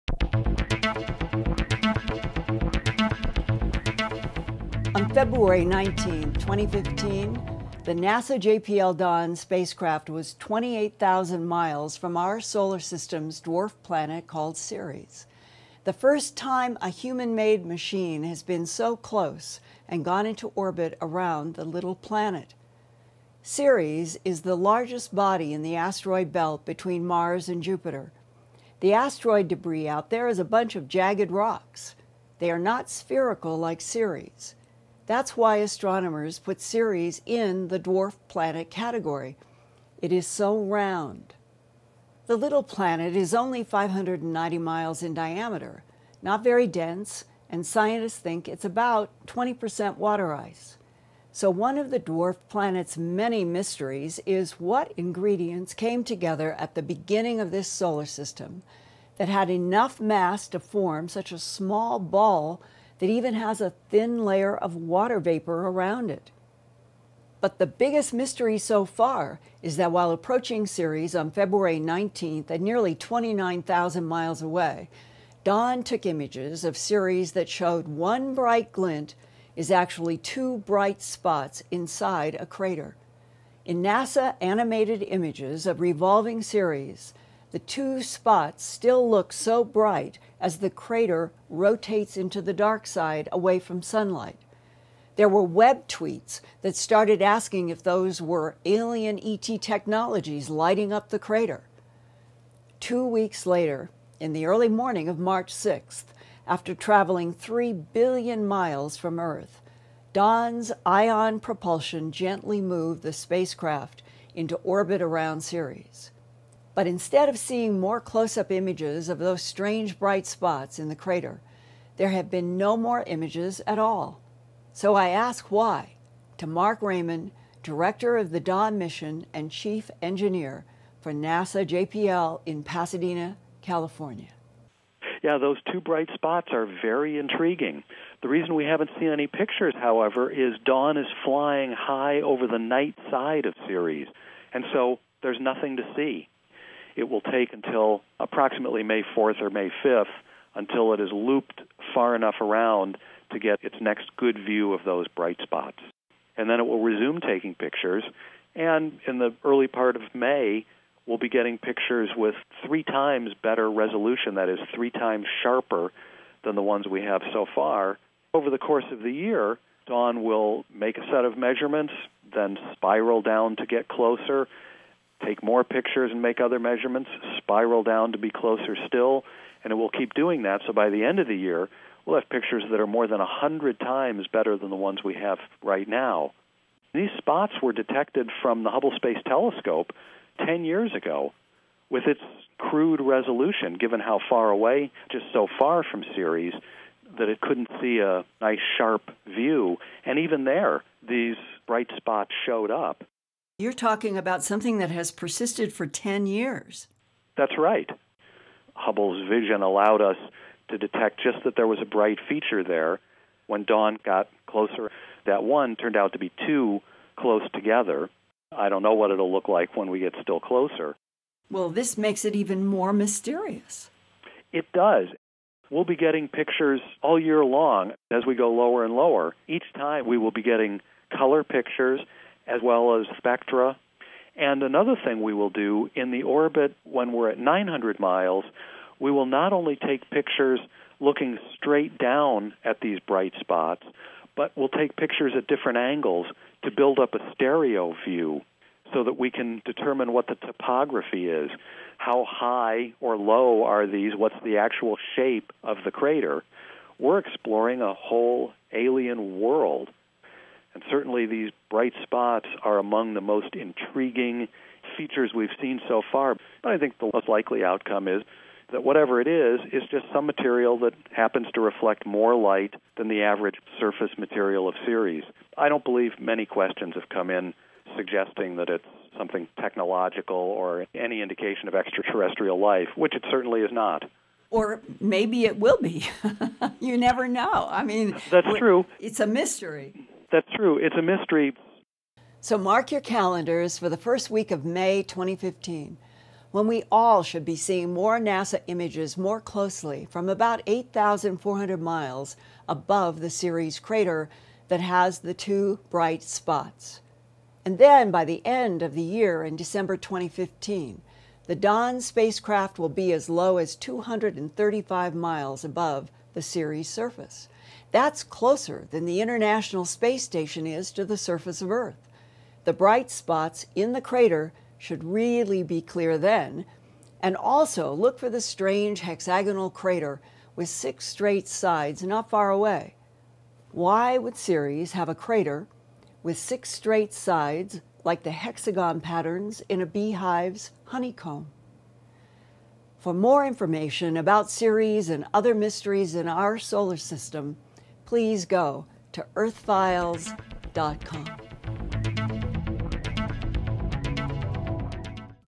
Earthfiles News April 3, 2015: Ceres- Mystery Bright Spots in Crater
The biggest mystery so far is that while approaching Ceres on February 19th at nearly 29,000 mile away, Dawn took images of Ceres that showed one bright glint from further away is actually two bright spots inside a crater.  In NASA-animated images of revolving Ceres, the two spots still look so bright as the crater rotates into the dark side away from sunlight.  Web tweets started asking if those were alien E. T. technologies lighting up the crater?